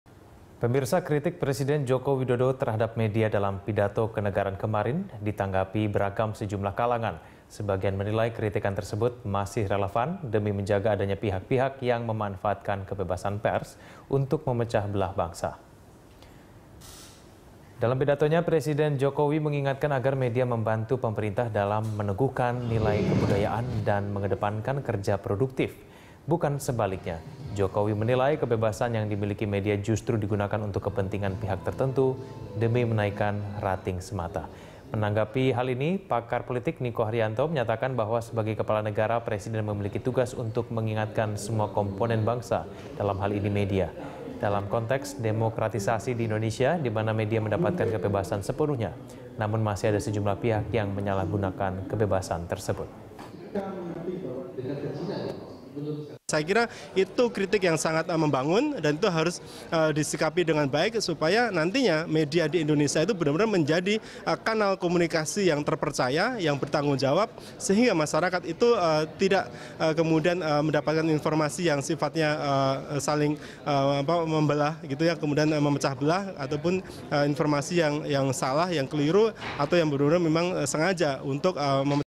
Kritikan Jokowi pada Media Harus Disikapi Obyektif
Kritik Presiden Joko Widodo terhadap media dalam pidato kenegaraan kemarin ditanggapi beragam sejumlah kalangan. Sebagian menilai kritikan tersebut masih relevan demi menjaga adanya pihak-pihak yang memanfaatkan kebebasan pers untuk memecah belah bangsa.

Facebook.com/BeritaSatuTV
Youtube.com/BeritaSatu